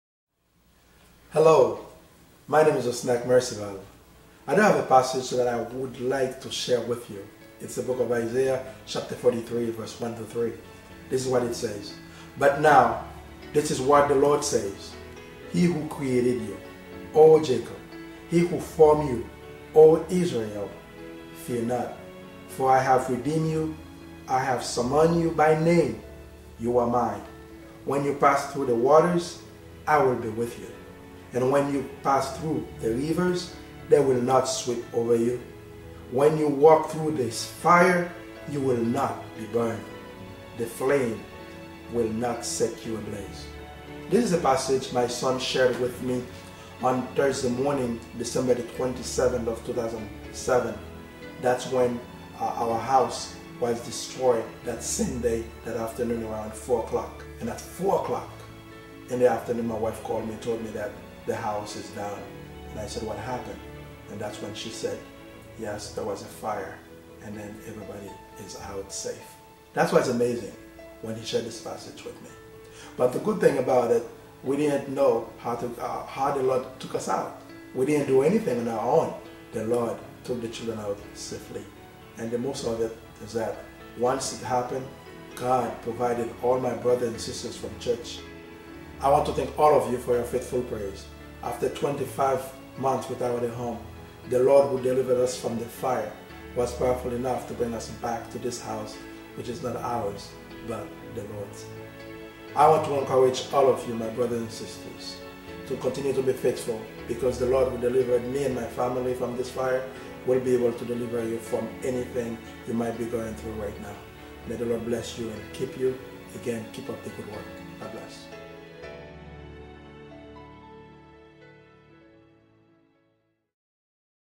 Osnak Mercival House Fire.mp4
On December 27th, 2007 around 3:00 in the afternoon Osnak mercival's home was destroyed by fire. Earlier that morning his son, Onyxn, read a scripture from his morning devotional from the book of Isaiah, chapter 43. In the third verse he read "When you walk through the fire, you will not be burned; the flames will not set you ablaze."
God delivered his family from the fire, then 25 months later, delivered the family back to their home. Their faith in God kept them together, and their prayers were answered.
Just another example of God revealing Himself to us in times of great trials.